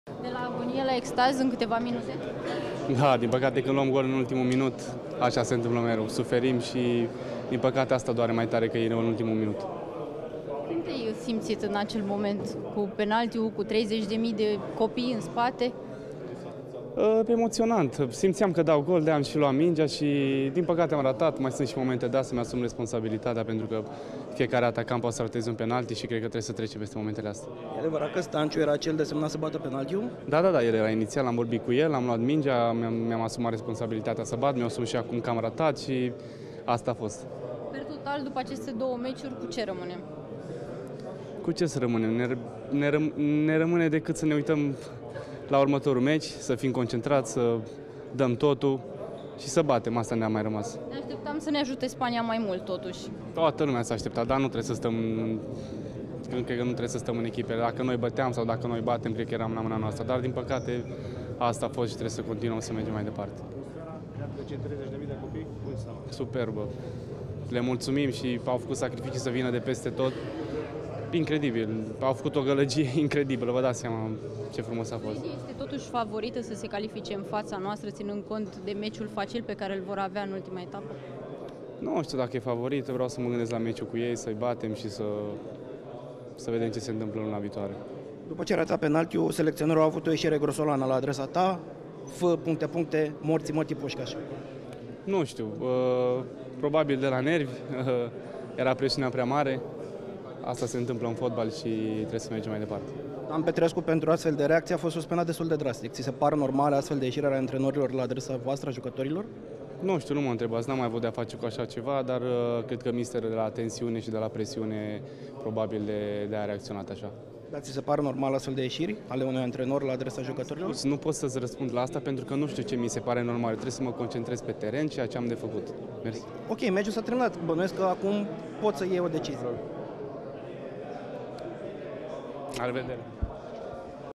Cosmin Contra, derapaj GROSOLAN la adresa lui George Pușcaș: "F***-ți morții mă-tii!"
România a remizat cu Norvegia, scor 1-1, în preliminariile EURO 2020. George Pușcaș (23 de ani) a ratat o lovitură de la 11 metri, a fost înjurat de selecționerul Cosmin Contra, iar după meci a avut un schimb tensionat de replici și cu jurnaliștii. Conștient de situația ingrată în care se află, Pușcaș a încercat să „negocieze” cu jurnaliștii prezenți la „zona mixtă”.